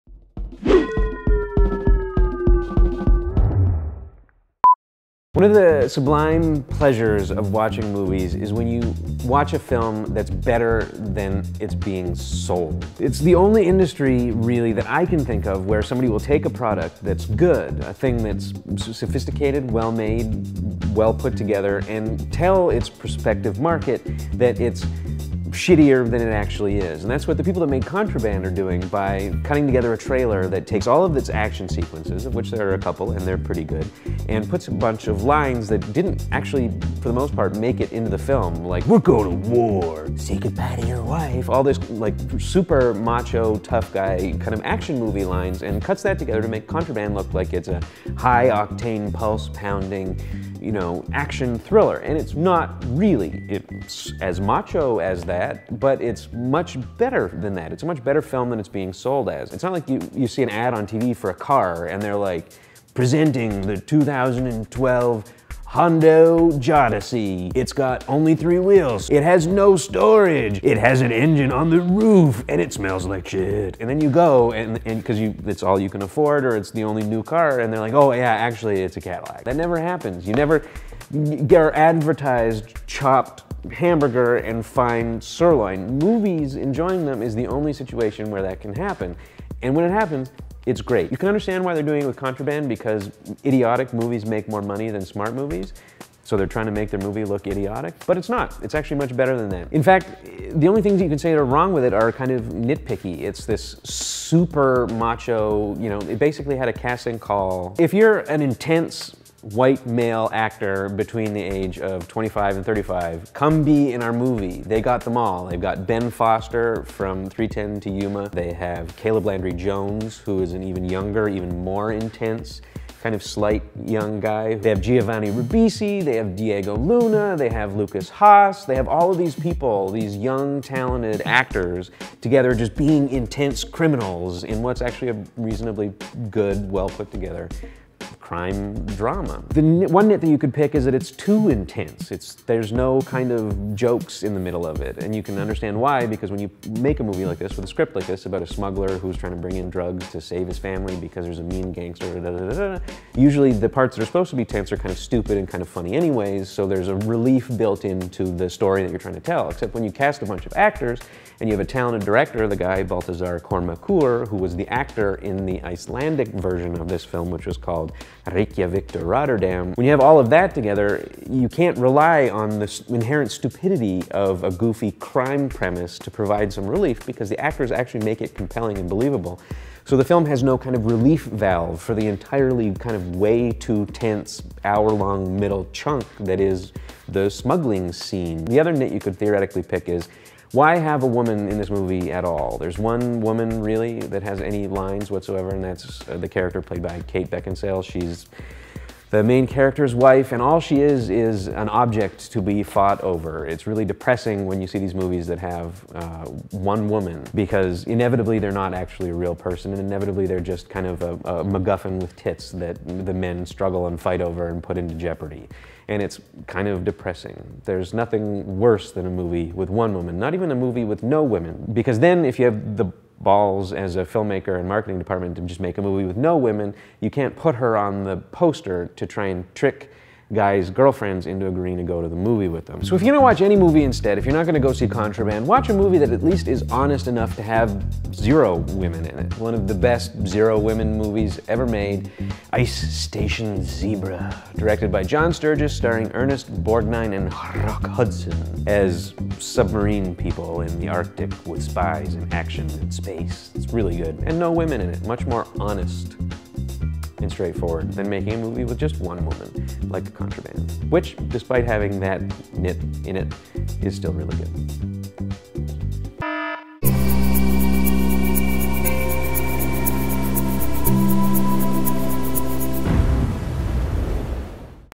WTI: Contraband
The latest Mark Wahlberg crime drama is less action-ey and stupid than the trailers might suggest, which is great because it meant that Mike was pleasantly surprised by Contraband, a film he had written off as probably terrible. It's not without it's pick-worthy nits, though, like the fact that there's very little in the way of comic relief and the obligatory inclusion of Kate Beckinsale who seems to only exist in this movie so they could put a pretty lady on the poster. That aside, it's worth checking out. And so is Mike's WTI suggestion starring Rock Hudson and Ernest Borgnine...